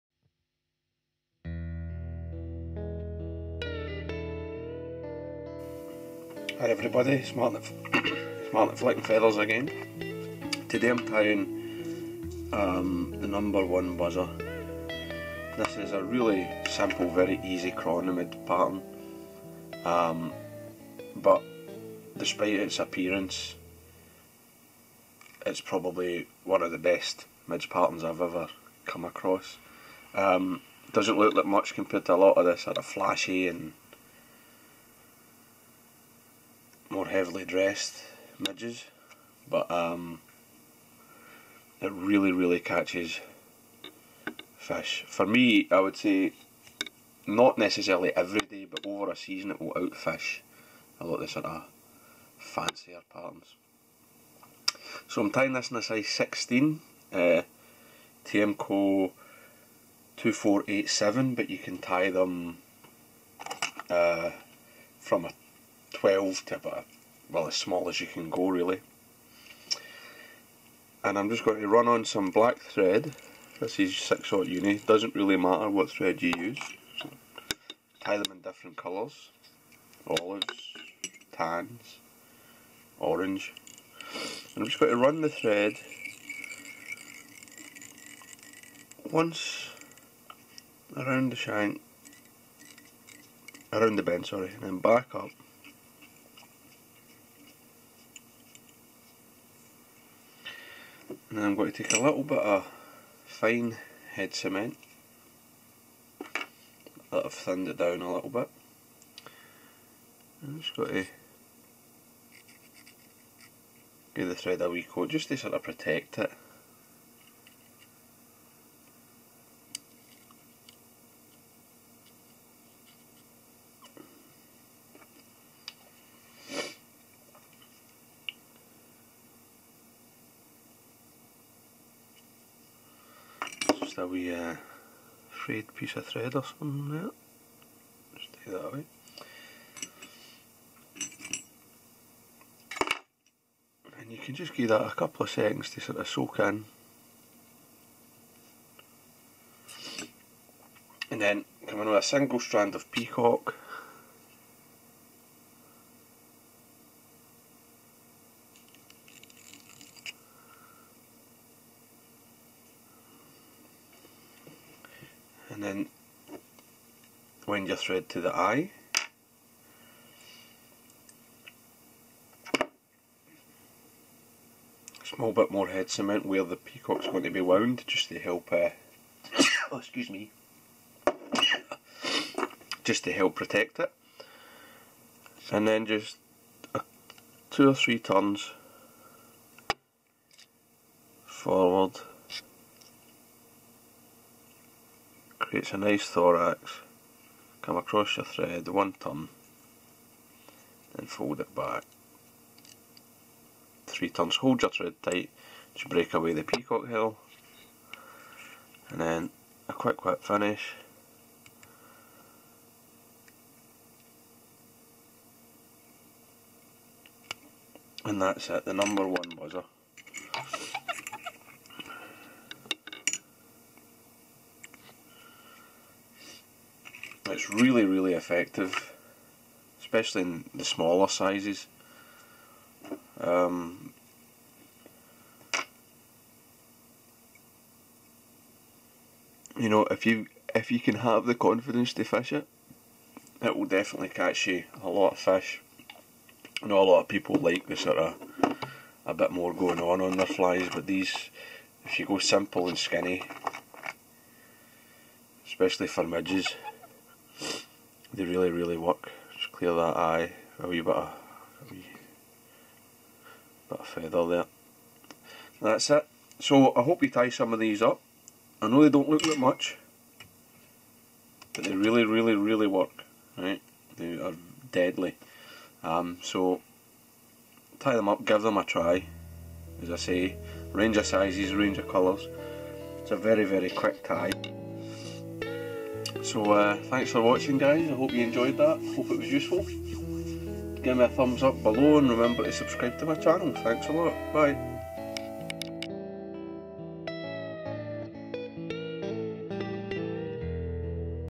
Tying the Number 1 Buzzer
Step by step instructions for a super easy, but very effective midge pattern.  Although it doesn't look like much, this simple buzzer will generally out fish more elaborate and gaudy imitations.
Materials Used
Hook: Kamazan B100/Tiemco 2487 size 12-22
Thread: 6/0 for larger sizes, 8/0 for smaller sizes
Thorax: peacock herl